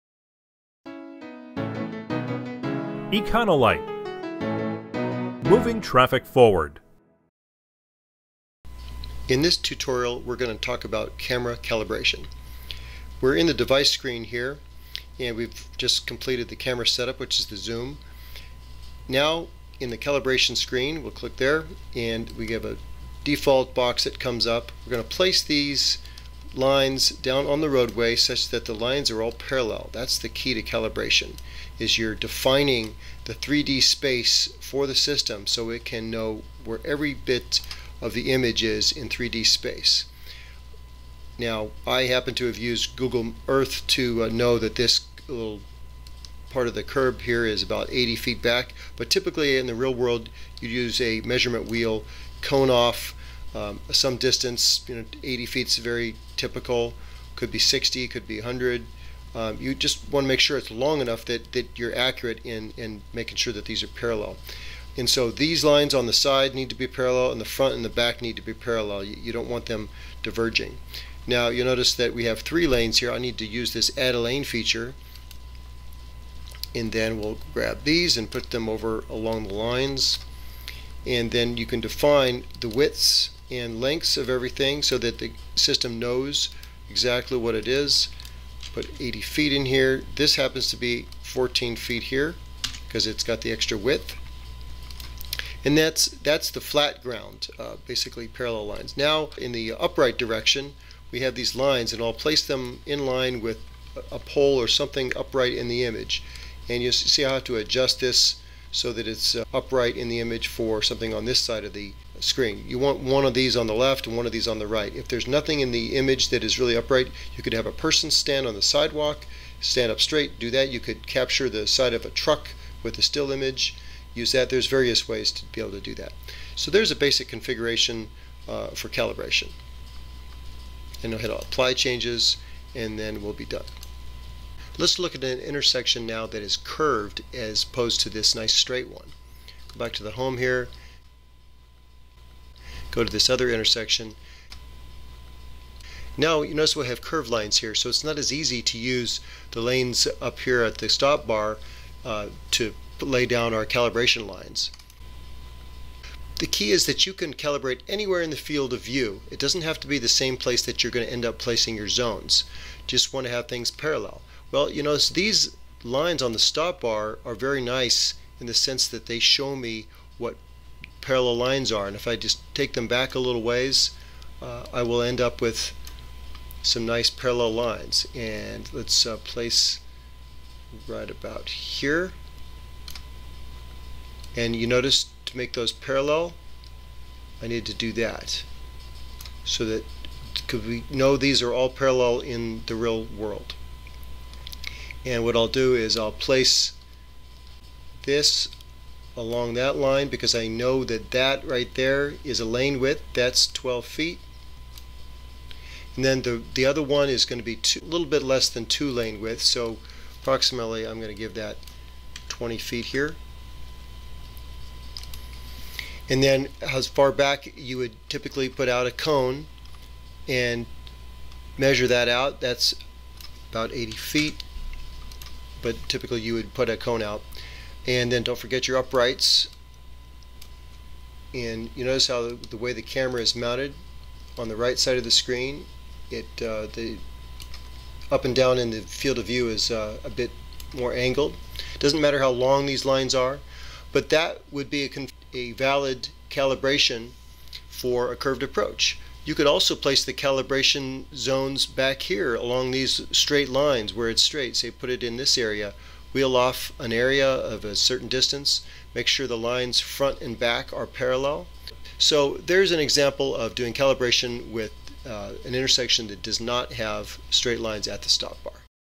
Autoscope Vision 2019 Tutorial IV - Camera Calibration
In this video, users learn how to calibrate zones and zoom into appropriate areas with Autoscope Vision.